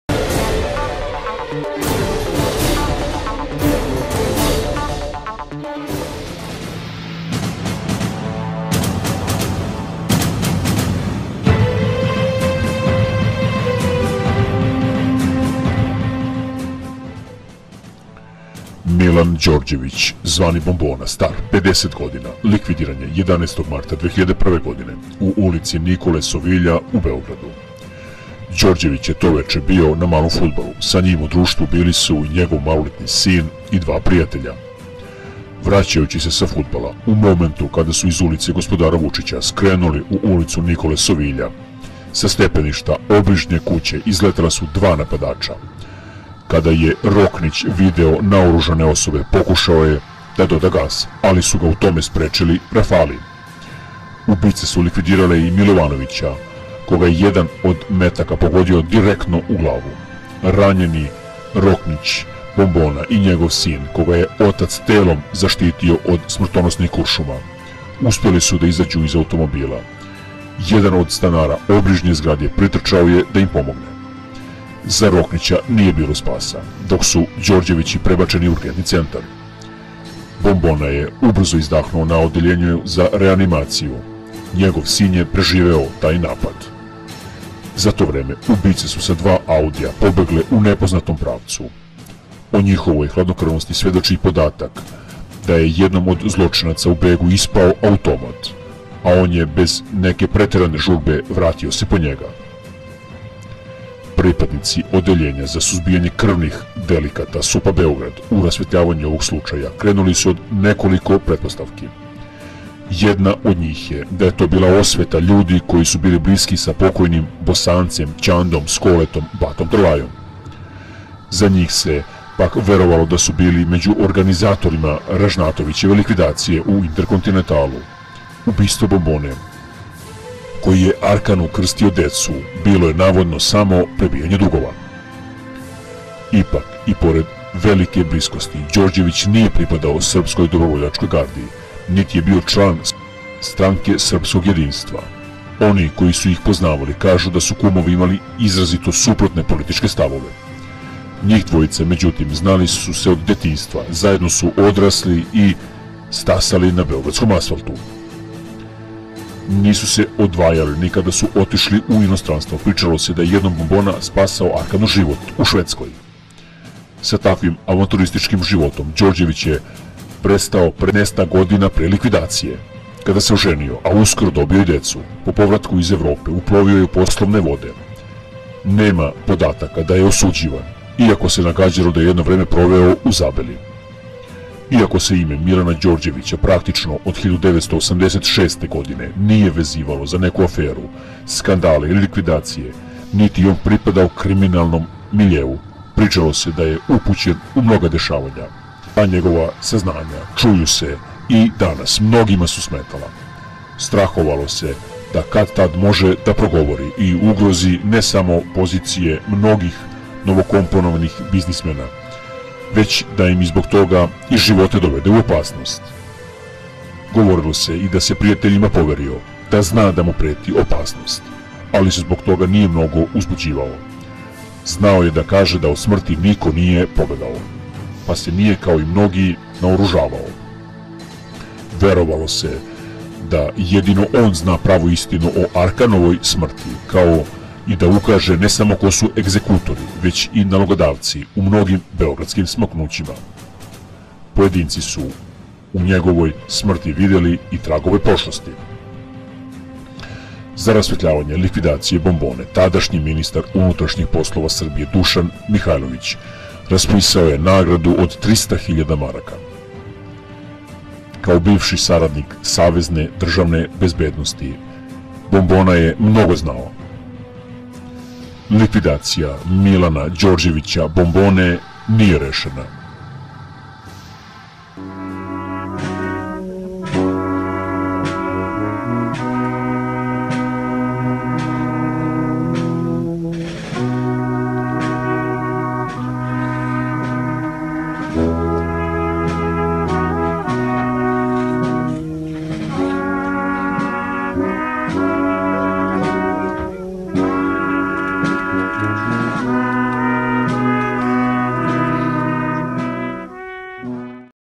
MILAN ĐORĐEVIĆ-BOMBONA(50) 11.03.2001
Milan Đorđević Bombona (50) likvidiran je 11 marta 2001 godine u ulici Nikole Sovilja,u Beogradu.
Đorđević je to veče bio na malom fudbalu,sa njim u društvu bili su njegov maloletni sin I dva prijatelja.
Vraćajuči se sa fudbala,U momentu kad su iz Ulice gospodara Vučića skrenuli u Ulicu Nikole Sovilja, sa stepeništa obližnje kuće izletela su dva napadača. Kada je Roknić video naoružane osobe pokušao je da doda gas, ali su ga u tome sprečili rafali. Ubice us likvidirale i  Milovanovića, koga je jedan od metaka pogodio direktno u glavu. Ranjeni Roknić, Bombona i njegov sin (koga je otac telom zaštitio od smrtonosnih kuršuma) uspeli su da izađu iz automobila. Jedan od stanara obližnje zgrade pritrčao je da pomogne.
Za Roknića nije bilo spasa, dok su Đorđevići prebačeni u Urgentni centar. Bombona je ubrzo izdahnuo na Odeljenju za reanimaciju,njegov sin je preživeo taj napad. Za to vreme ubice su sa dva **audija** pobegle u nepoznatom pravcu. O njihovoj hladnokrvnosti svedoči i podatak da je jednom od zločinaca u begu ispao automat, a on se bez neke preterane žurbe  vratio po njega.
Pripadnici Odeljenja za suzbijanje krvnih delikata SUP Beograd u rasvetljavanju ovog slučaja krenuli su od nekoliko pretpostavki. Jedna od njih je i da je to osveta ljudi koji su bili bliski sa pokojnim Bosancem, Ćandom, Skoletom, Batom Trlajom... Za njih se, pak, verovalo da su bili među organizatorima Ražnatovićeve likvidacije u **Interkontinentalu**. Ubistvo Bombone, koji je Arkanu krstio decu   bilo je, navodno, **prebijanje dugova**.
 IPAK, i pored velike bliskosti, Đorđević nije pripadao Srpskoj dobrovoljačkoj gardi, niti je bio član Stranke srpskog jedinstva. Oni koji su ih poznavali kažu da su kumovi imali izrazito suprotne političke stavove. NJih dvojica, međutim, znali su se od detinjstva, zajedno su odrasli i stasali na beogradskom asfaltu.
Nisu se odvajali ni kad su otišli u inostranstvo. Pričalo se da je jednom Bombona spasao Arkanu život u Švedskoj. Sa takvim, avanturističkim životom, Đorđević je prestao pre petnaestak godina, kada se oženio, a uskoro dobio i decu. Po povratku iz Evrope uplovio je u poslovne vode. Nema podataka da je osuđivan, iako se nagađalo da je jedno vreme proveo u Zabeli.
Iako se ime Milana Đorđević, praktično od 1986. godine, nije vezivalo za neku aferu, skandal ili likvidaciju, niti je on pripadao kriminalnom miljeu, pričalo se da je upućen u mnoga dešavanja. Ta njegova saznanja, čuje se danas, mnogima su smetala. Strahovalo se da kad-tad može da progovori i ugrozi ne samo pozicije mnogih **novokomponovanih biznismena**, već da im zbog toga i živote dovede u opasnost.
Govorilo se i da je prijateljima poverio da zna da mu preti opasnost, ali se zbog toga nije mnogo uzbuđivao. Znao je da kaže da od **smrti niko nije pobegao**, pa se nije, kao mnogi, ni naoružao. Verovalo se da jedino on zna pravu istinu o Arkanovoj smrti, kao i da ukaže ne samo ko su egzekutori, već i nalogodavci u mnogim beogradskim smaknućima.
Pojedinci su u njegovoj smrti videli i tragove prošlosti.
Za rasvetljavanje likvidacije Bombone, tadašnji ministar unutrašnjih poslova Srbije Dušan Mihajlović raspisao je nagradu od 300.000 maraka.
Kao bivši saradnik Savezne DB mnogo toga je znao.Likvidacija Milana Đorđevića Bombone nije rešena.